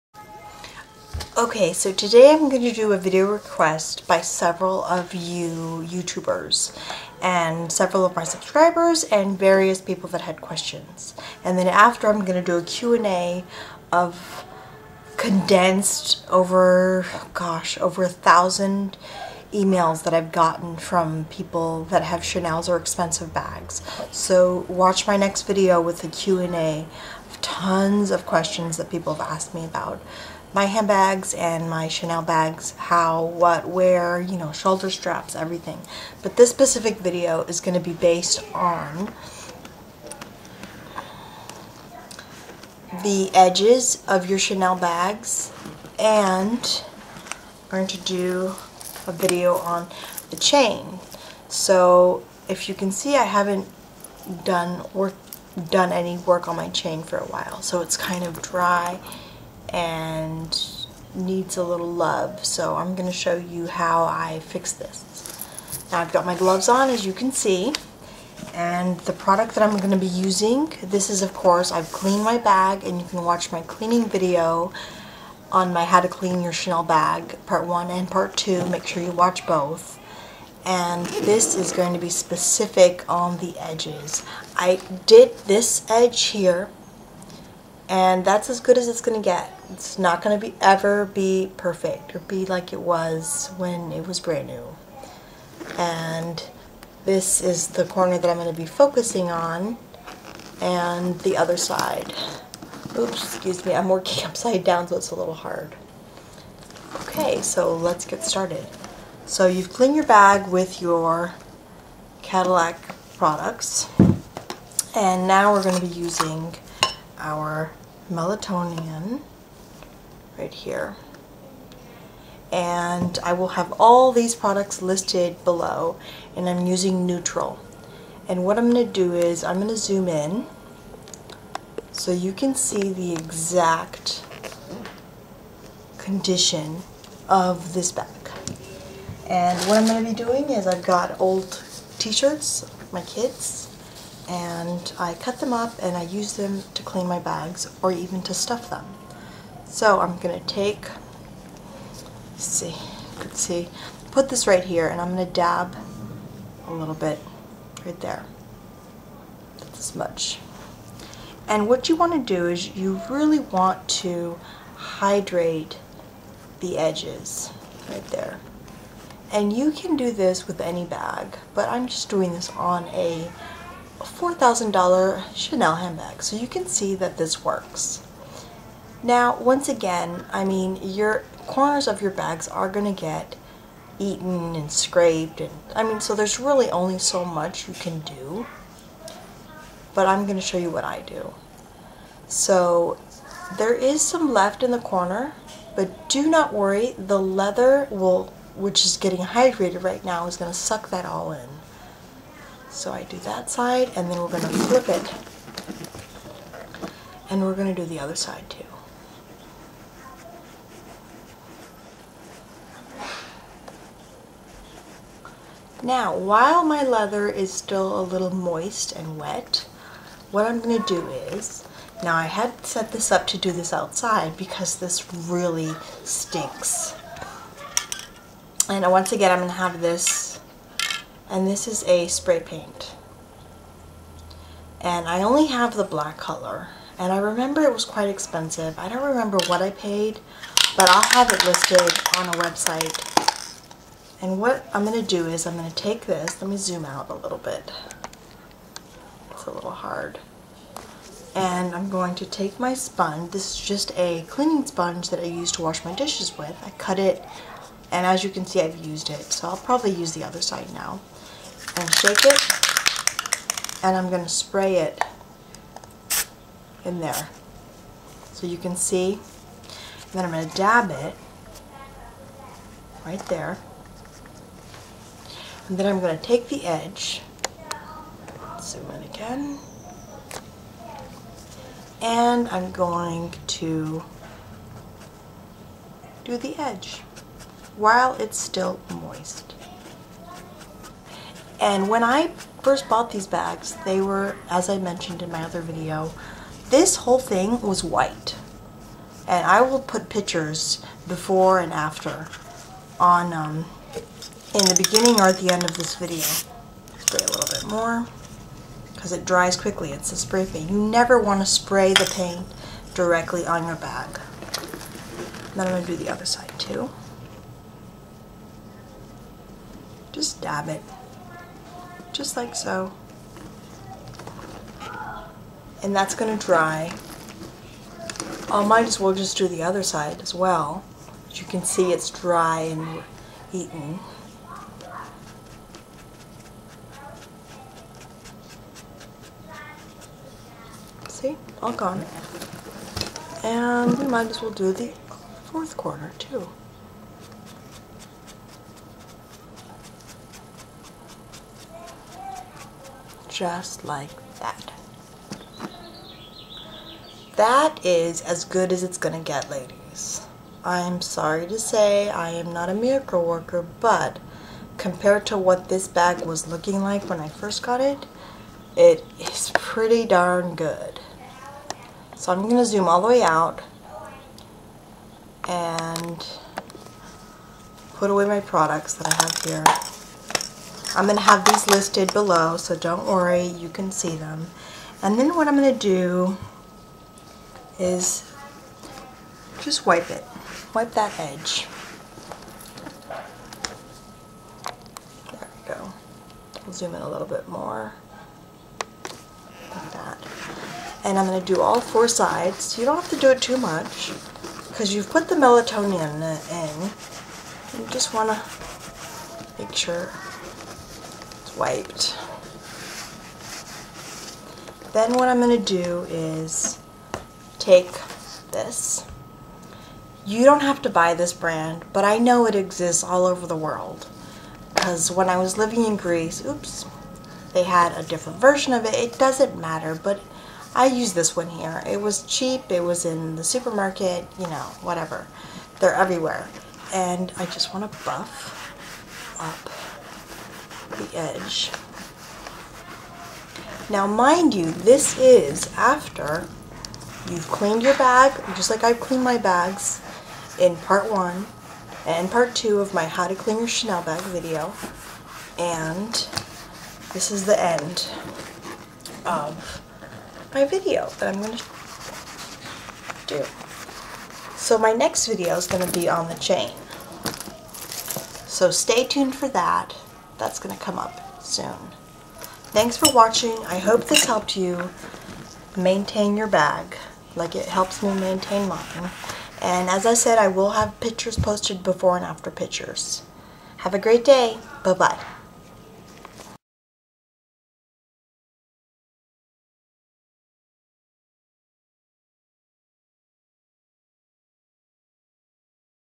How to Repair Your Chanel Bag Corners
Here are links to where you can buy the products I used in my videos.
This link is to websites that ship worldwide. I am sure if you Google the products for USA residents you will find them cheaper. Both products can be bought at shoe repair stores.

 Just discovered this one. It is as good or maybe even better than the one that I used in my video.

Hope this helps YALL!!